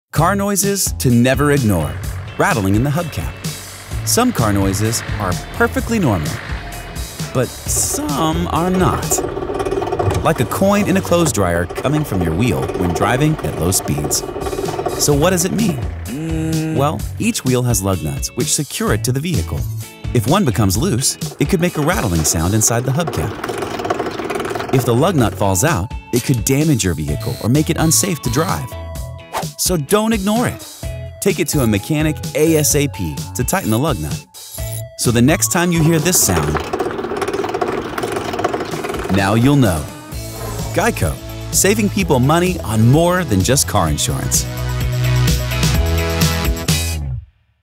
How To Fix A Rattling Hubcap - GEICO Insurance
When your tires' hubcaps are rattling, a lugnut may be loose that could cause damage to your car.

GEICO's been saving people money for over 75 years.